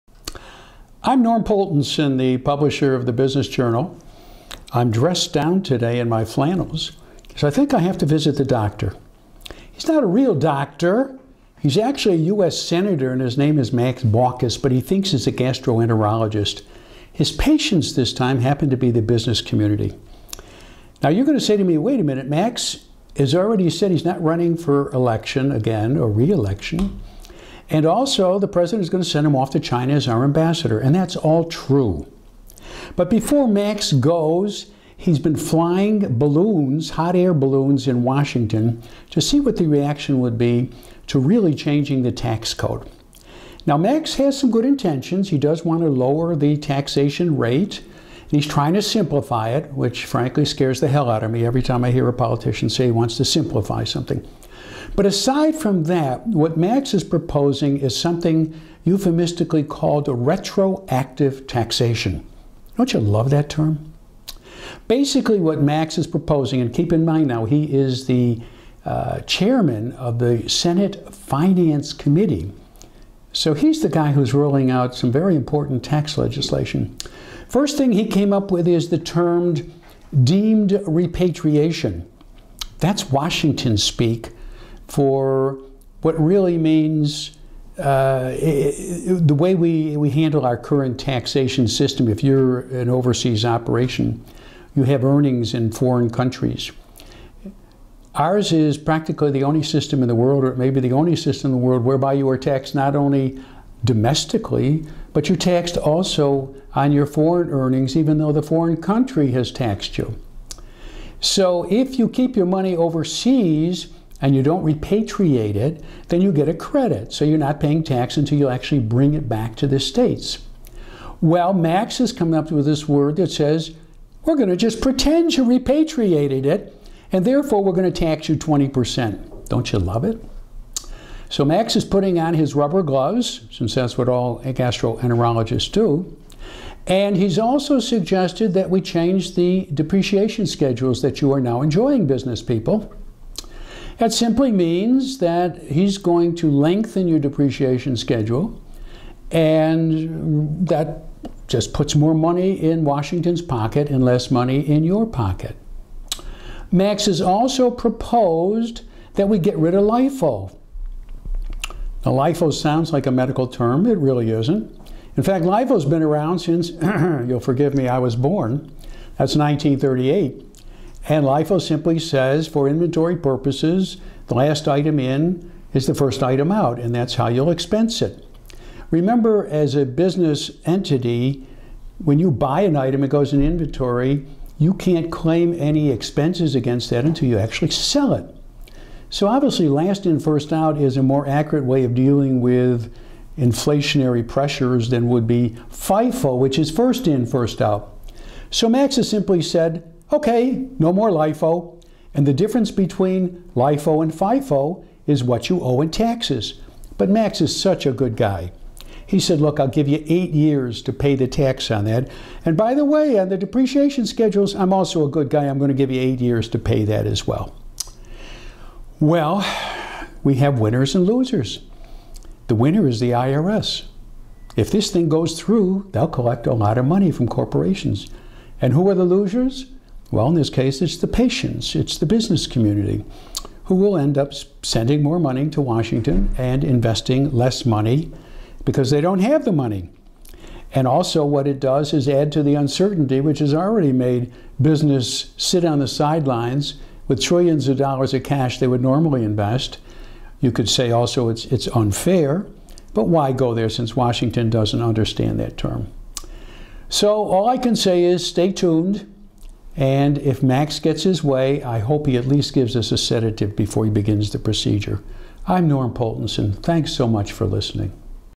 Norm Poltenson on retroactive taxation
Norm Poltenson, publisher of The Central New York Business Journal, discusses his latest opinion article on Senator Max Baucus's proposal for retroactive taxation on the business community. For more, see his opinion column in the Dec. 20-27, 2013 issue of The Business Journal.